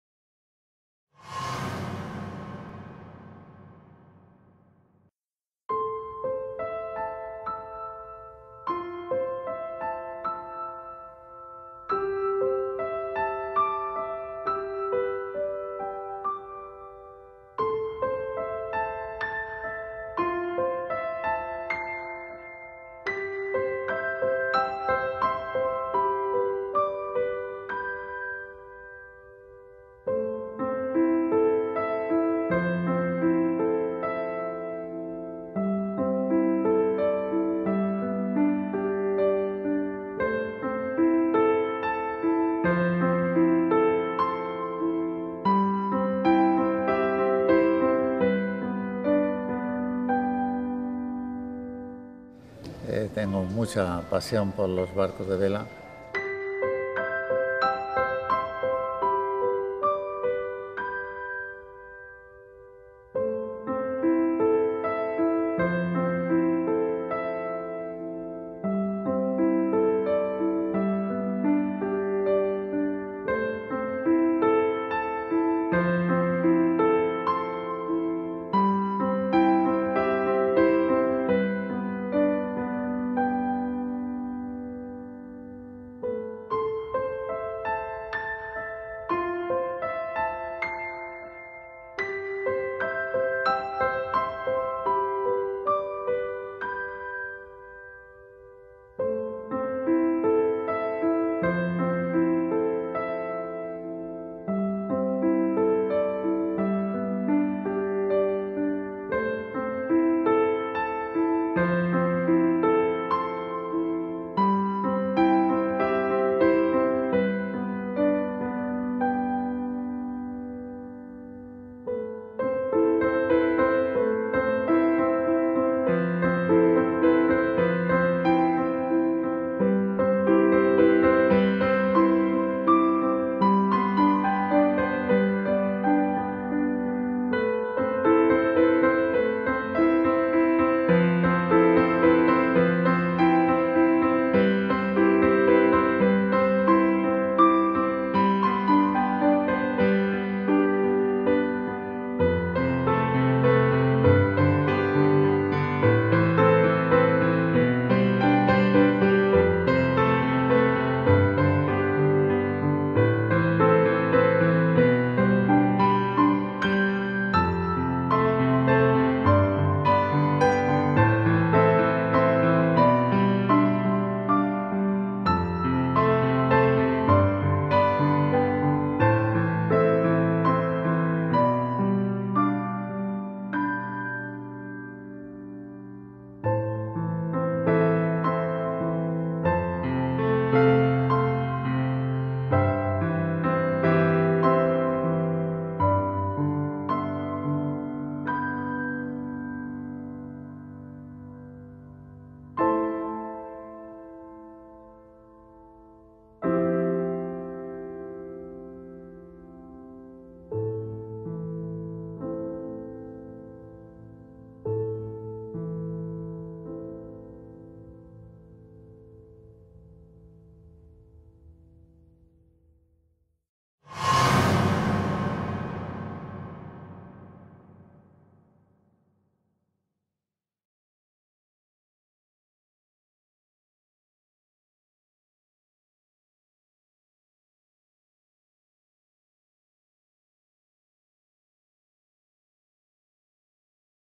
The New Belliure B50 SY sailboat
After a 20+ year absence manufacturing sailboats, Belliure have begun building the new B50 SY. With the combination of the classic design skills of Vicente Belliure and the innovative Iñigo Toledo from Barracuda Yacht Design it looks to be a combination of luxury, quality design ( - what we have come expect from the Belliure craftsmen) and the innovative cutting edge design from Barracuda Yacht Design. The result, many in the sailing world are waiting expectantly for...
Here is a quick video montage of the first step in the construction process...